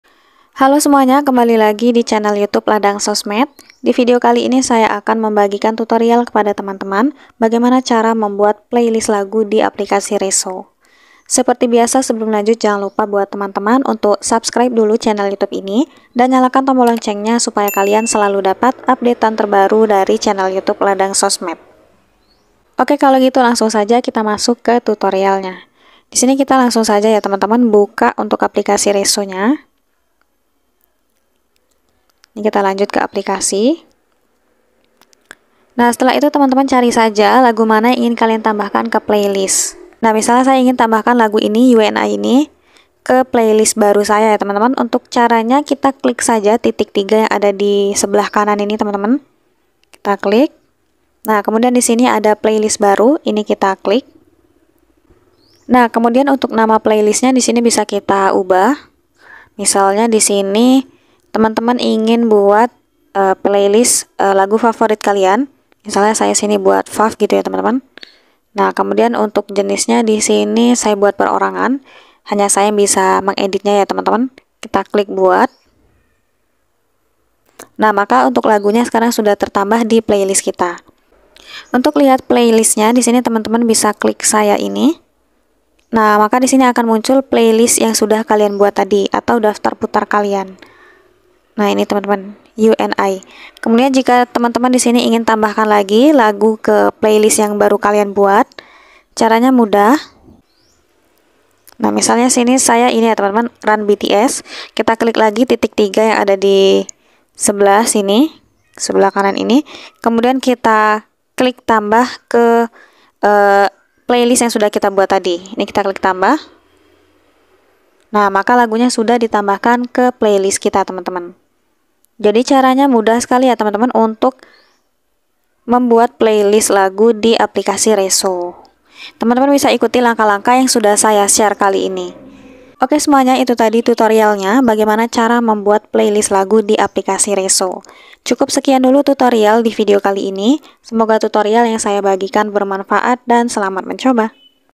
Cara Membuat Playlist Lagu di Resso
Fitur sosial Resso mengajakmu berinteraksi dengan teman lewat lagu favorit, membuat dan membagikan playlist, serta saling berkomentar tentang lagu yang kamu suka.